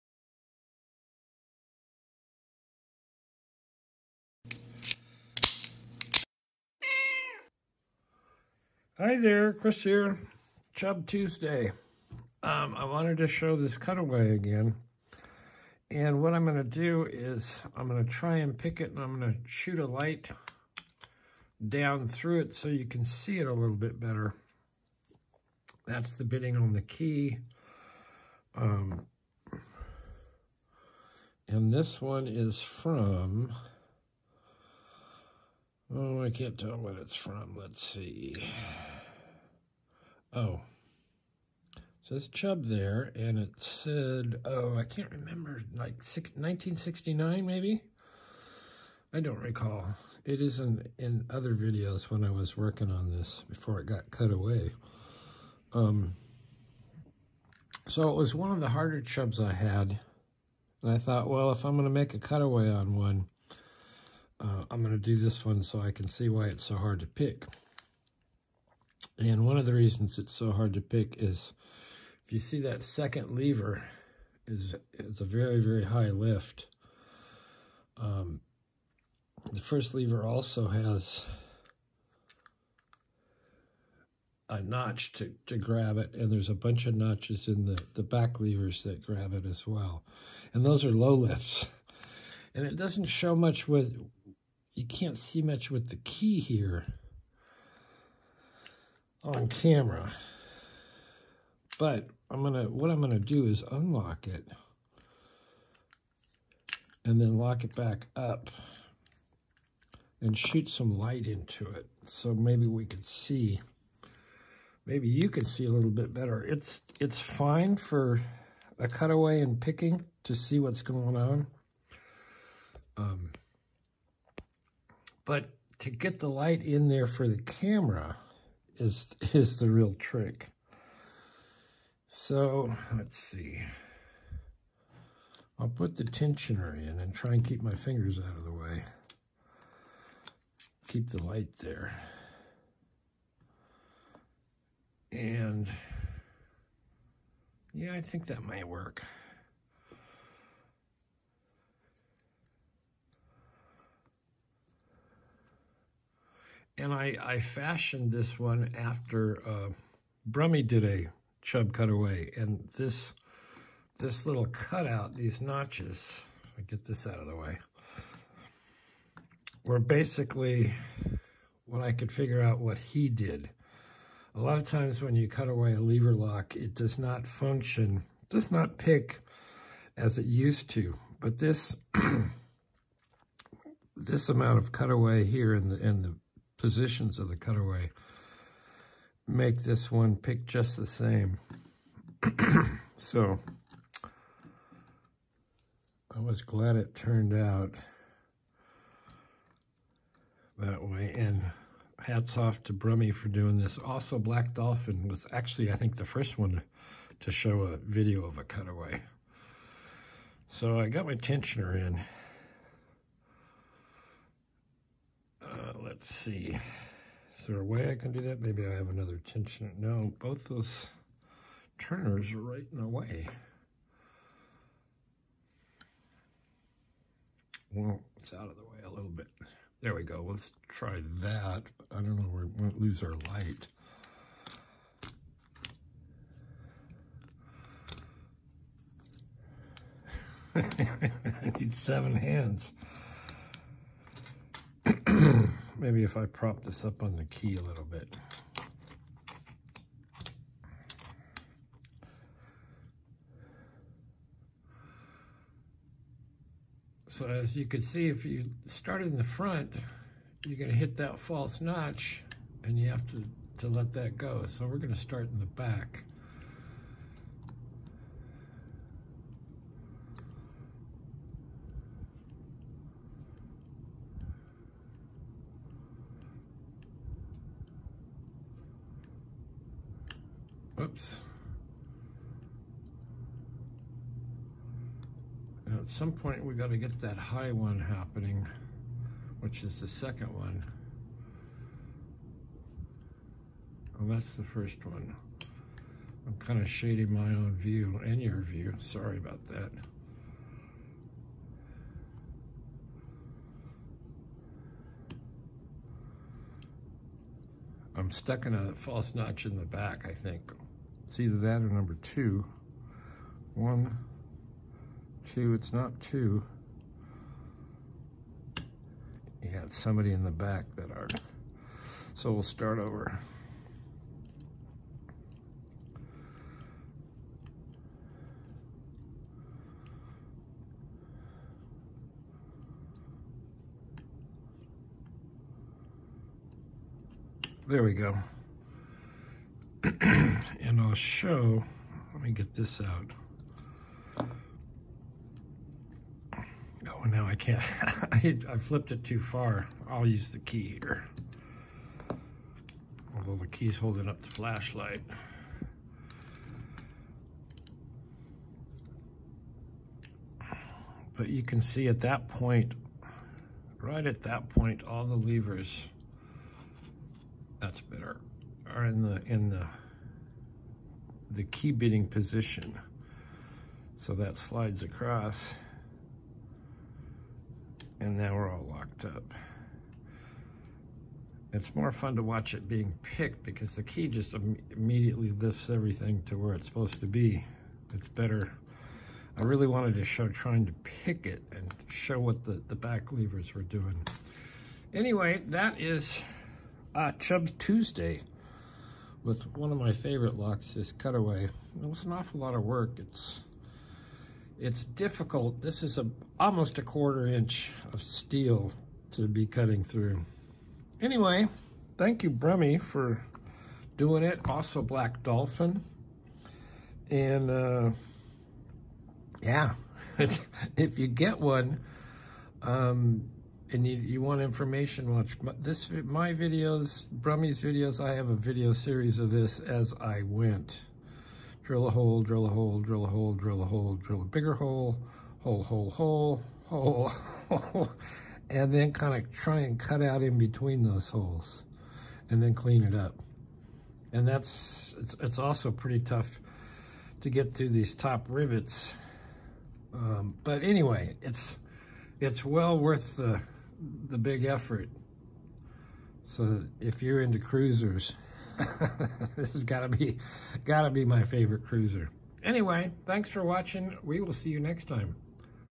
#1929 chubbtuesday
locksport Chubb Cruiser cutaway picked with back light